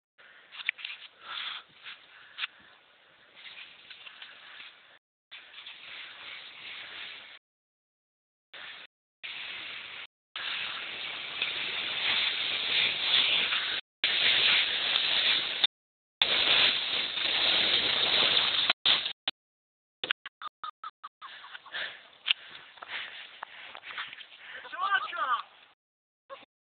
yfz450 Puerto Rico
My brother Carlos trying to climb up the "Wilfredo Hill" in La Cantera in Ponce, Puerto Rico.